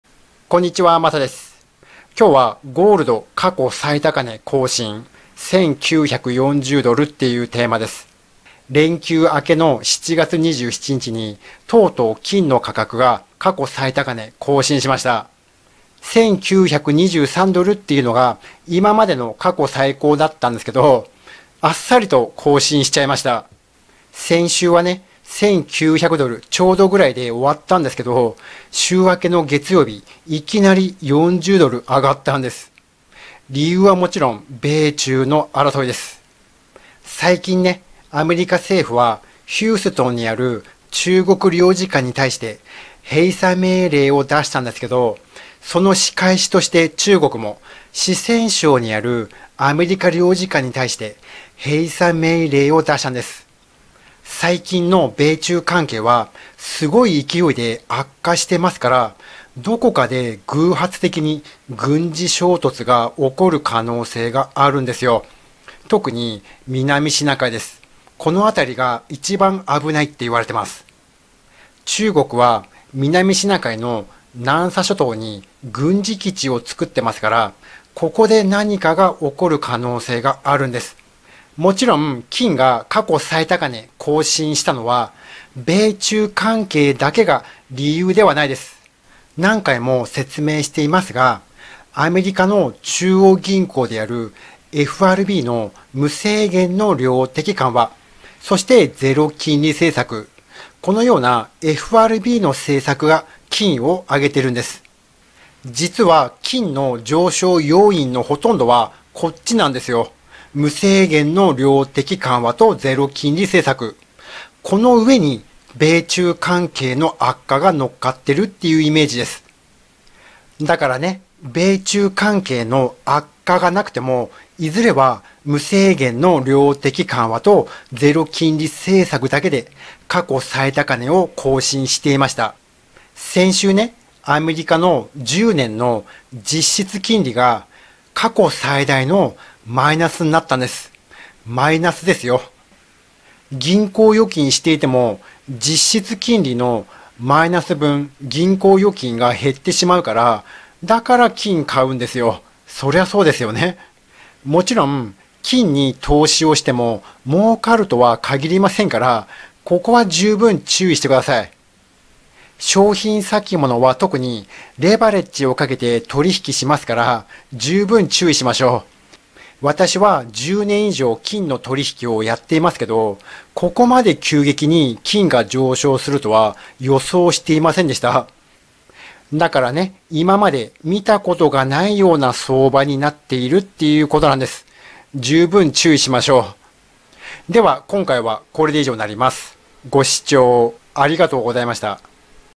ゴールドの価格が過去最高値更新！その理由は
ゴールドがとうとう1923ドルを超えてきました。私の予想よりもかなり早い